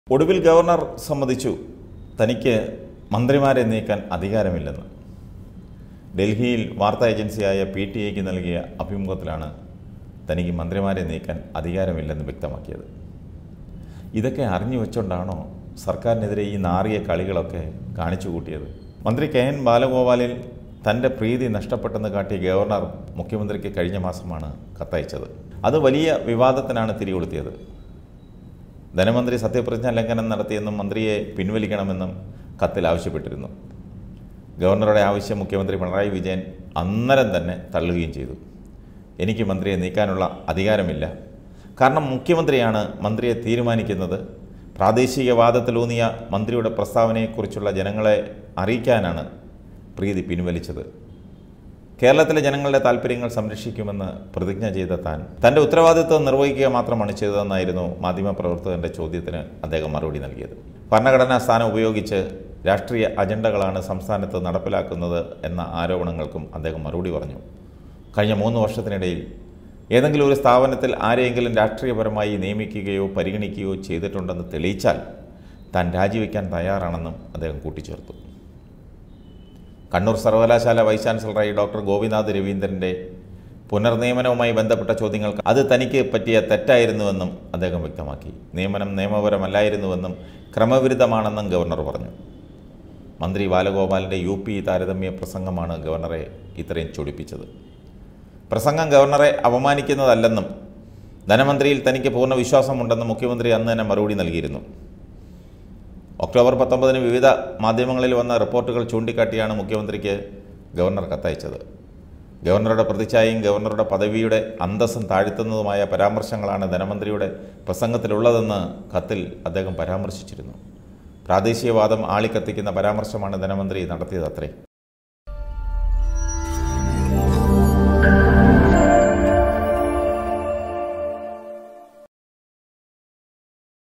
മന്ത്രിമാരെ നീക്കാൻ തനിക്ക് അധികാരമില്ലന്ന് ഗവർണർ സമ്മതിച്ചു
Tag-Generator-Youtube-malayalam news-global